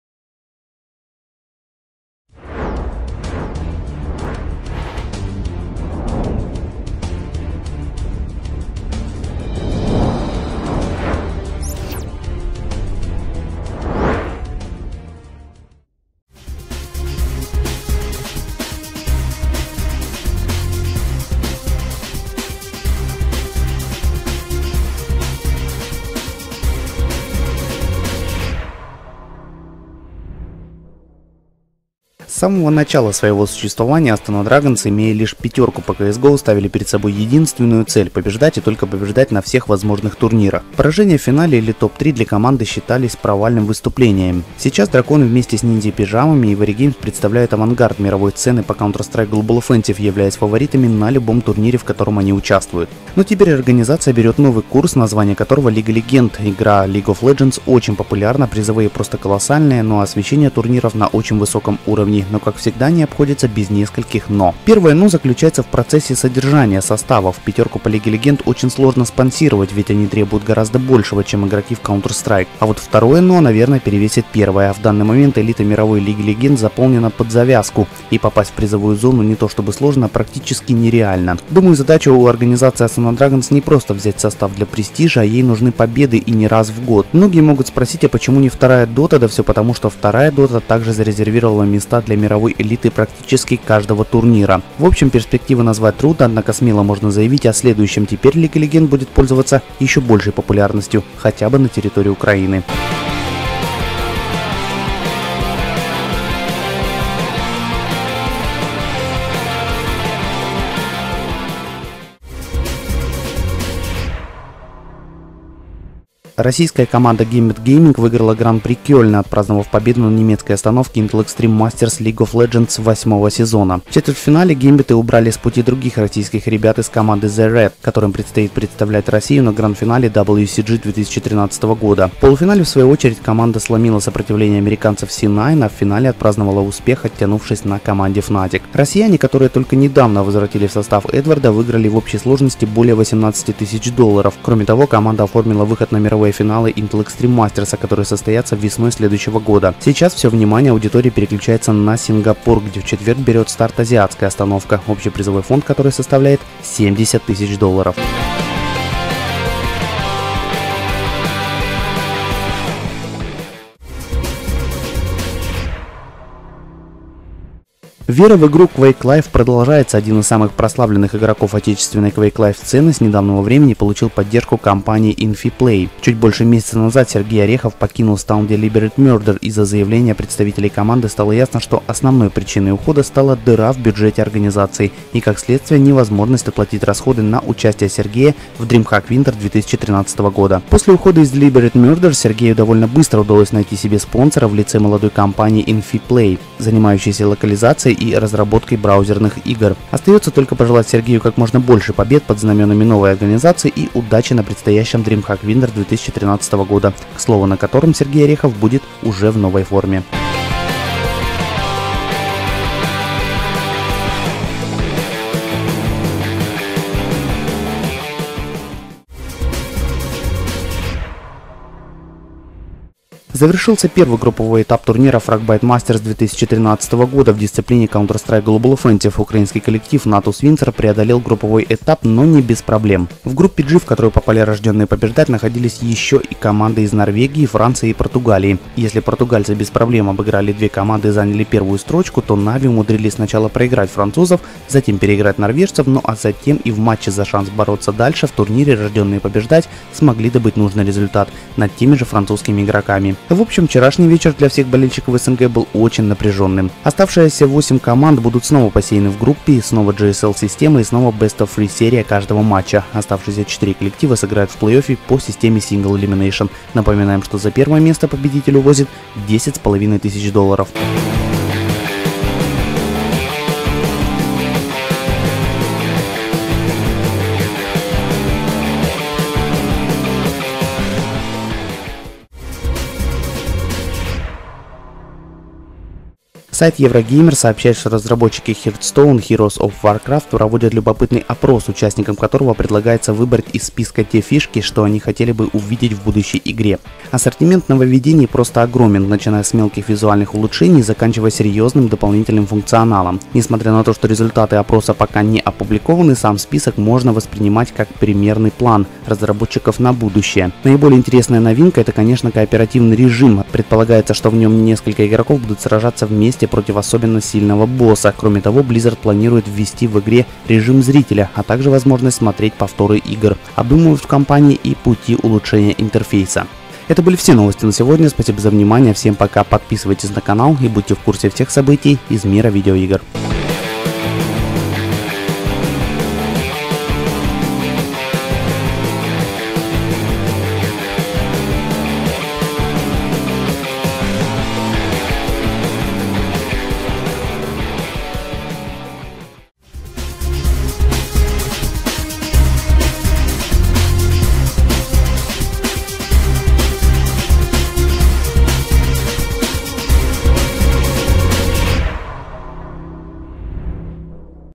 Astana Dragons и League Of legends -27-11-2013 - WES Cyber News
- Astana Dragons и League Of legends
- Gambit Gaming  выиграли IEM Кёльн
- INFIPLAY. evil
- Fragbite Masters 2013
- Hearthstone и "Новый режим"